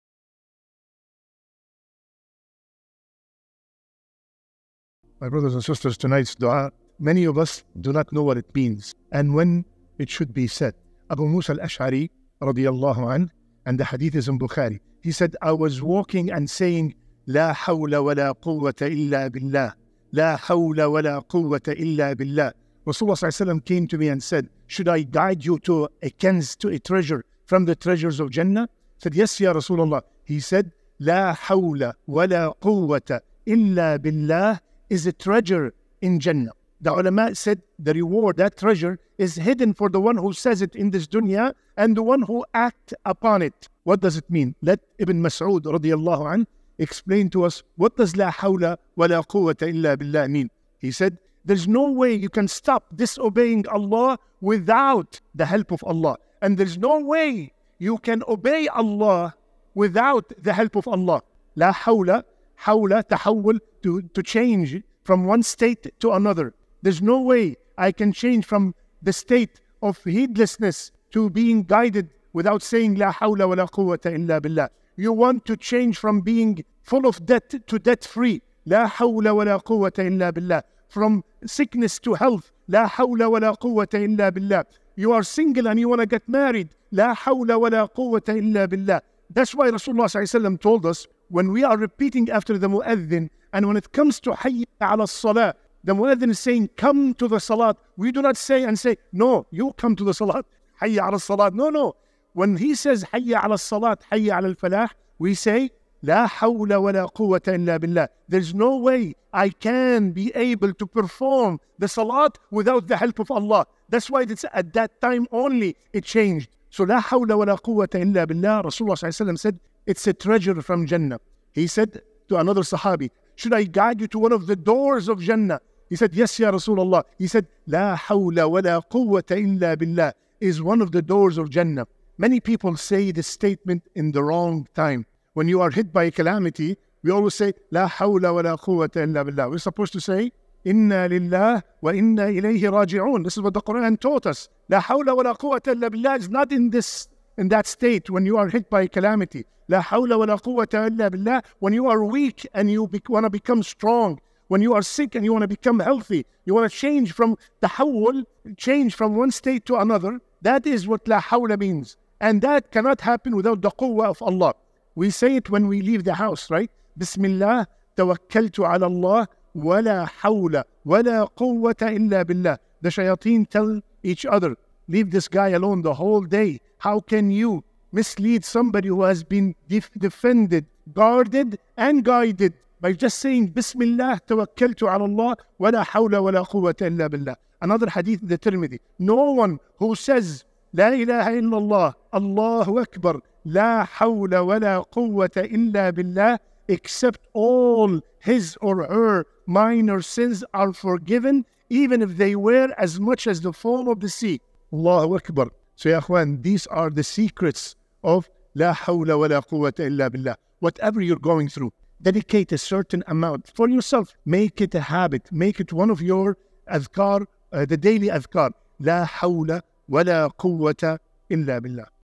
Say This Dua: To Transform Your Life
Discover the profound meaning of لا حَوْلَ وَلا قُوَّةَ إِلا بِالله and when to use it for positive change in your life. Join us as we explore the wisdom behind this powerful phrase and how it can transform your relationship with Allah. Learn when and how to say [Arabic] correctly and its significance in seeking Allah's help. 🤲✨

Timestamps:

00:00:00 - Introduction: The Treasure of لا حَوْلَ وَلا قُوَّةَ إِلا بِالله
00:00:30 - لا حَوْلَ وَلا قُوَّةَ إِلا بِالله Explained: A Door to Positive Change
00:01:30 - When to Say لا حَوْلَ وَلا قُوَّةَ إِلا بِالله: Correct Timing and Usage
00:02:30 - إِنَّا لِلَّٰهِ وَإِنَّا إِلَيْهِ رَاجِعُون in Times of Calamity vs. Strength
00:03:30 - The Power of لا حَوْلَ وَلا قُوَّةَ إِلا بِالله: Forgiveness of Sins
00:04:00 - Daily Practice: Incorporating لا حَوْلَ وَلا قُوَّةَ إِلا بِالله into Your Routine

Surah Al-Baqarah (2:286): "Allah does not burden a soul beyond its scope."